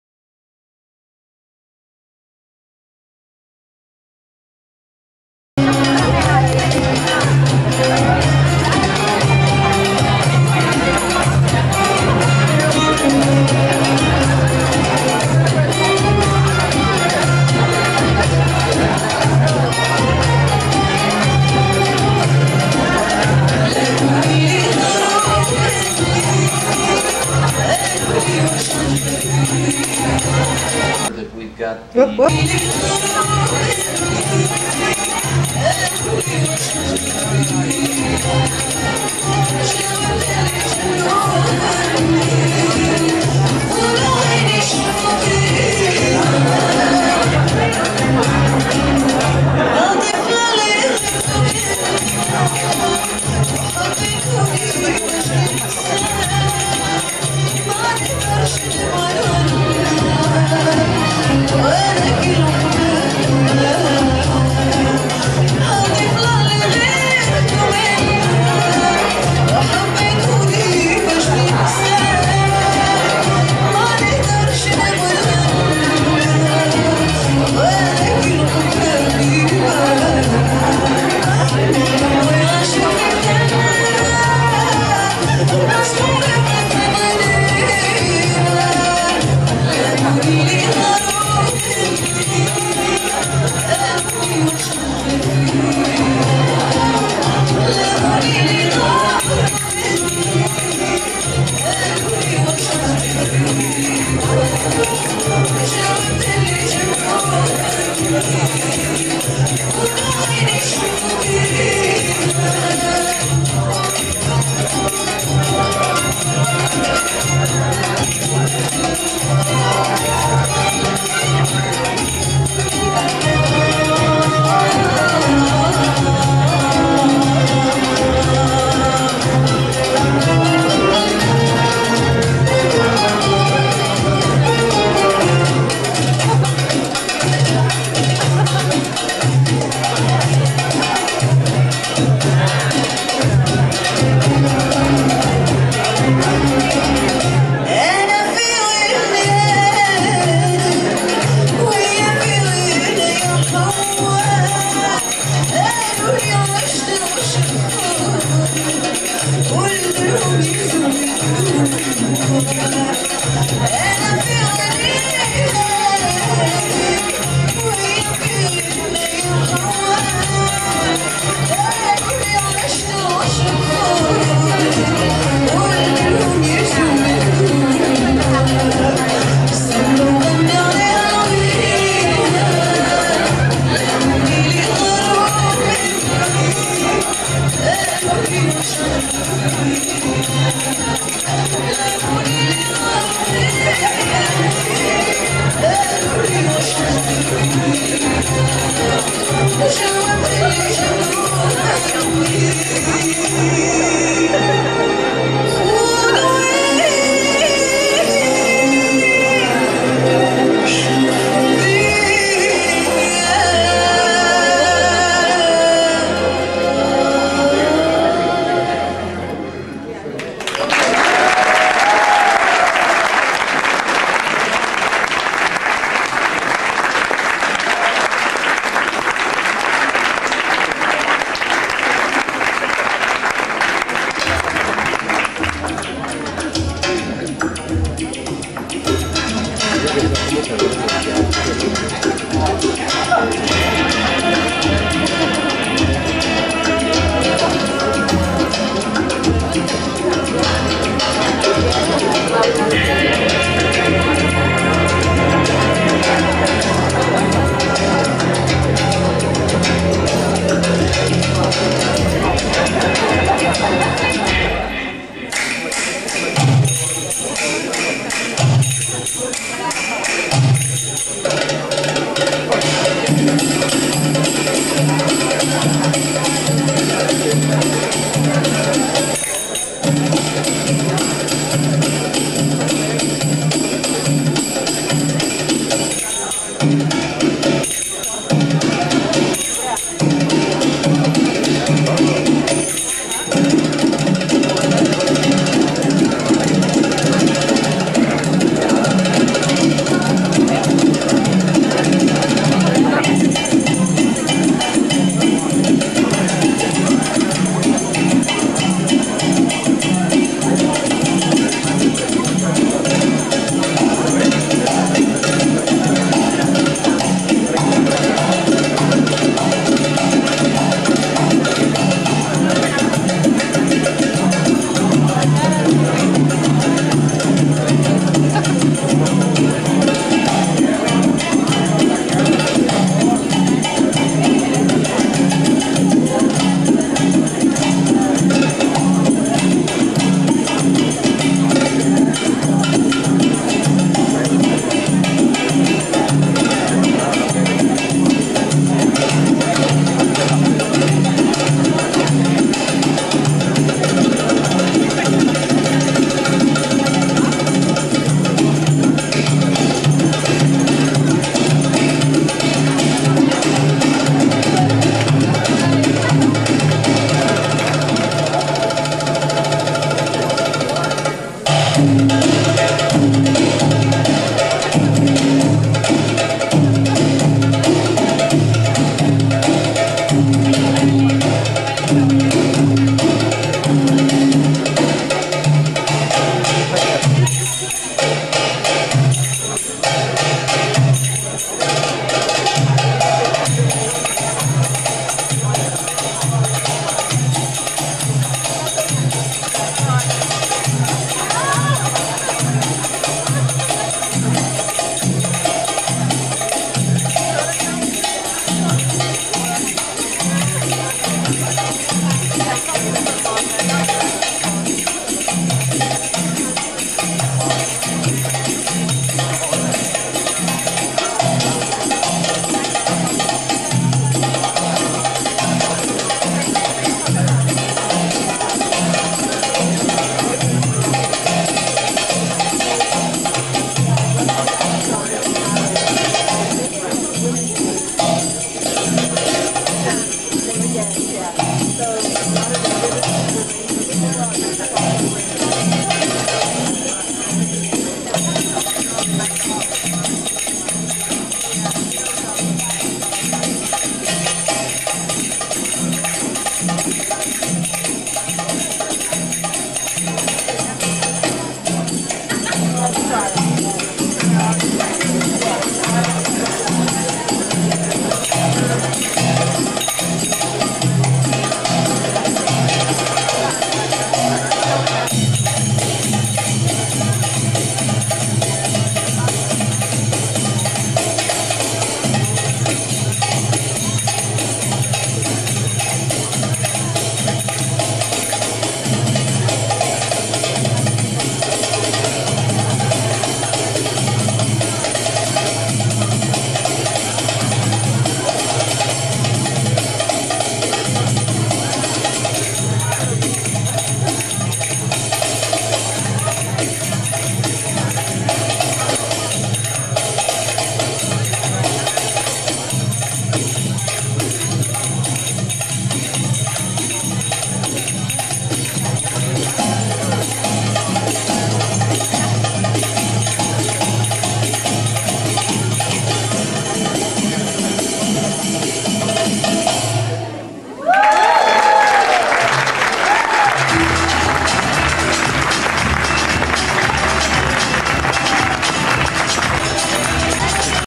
Bonita Brockert Belly Dance Performance, P & G International Night
Belly Dance with drum solo
Bonita Brockert show for P & G International party